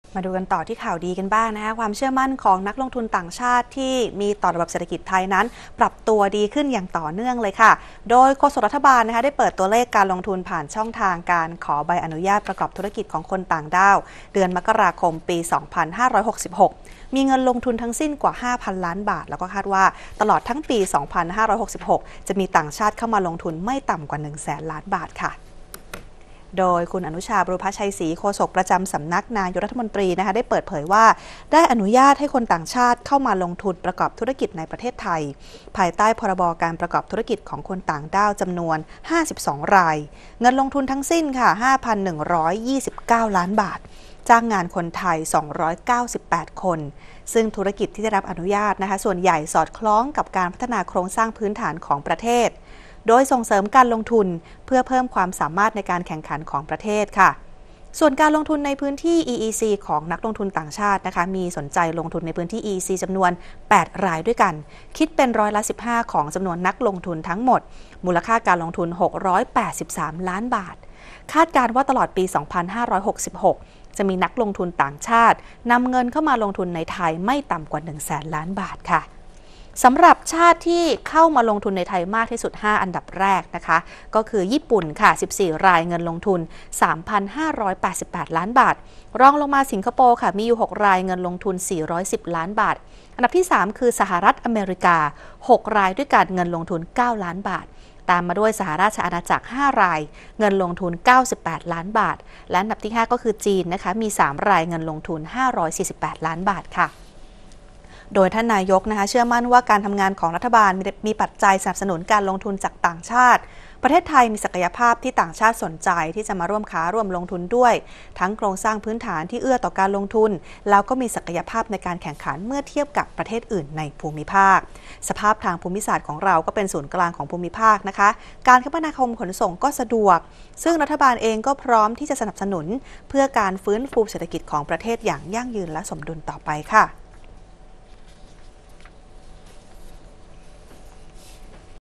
การลงทุนของคนต่างชาติในไทย เดือน ม.ค. กว่า 5 พันล้านบาท ข่าวเที่ยง วันที่ 6 มีนาคม 2566
ความเชื่อมั่นของนักลงทุนต่างชาติมีต่อระบบเศรษฐกิจไทยปรับตัวดีขึ้นอย่างต่อเนื่อง โดยโฆษกรัฐบาล เปิดตัวเลขการลงทุนผ่านช่องทางการขอใบอนุญาตประกอบธุรกิจของคนต่างด้าว เดือนมกราคม ปี2566 มีเงินลงทุนทั้งสิ้นกว่า5 พันล้านบาท คาดว่าตลอดทั้งปี2566 จะมีต่างชาติเข้ามาลงทุนไม่ต่ำกว่า 1 แสนล้านบาท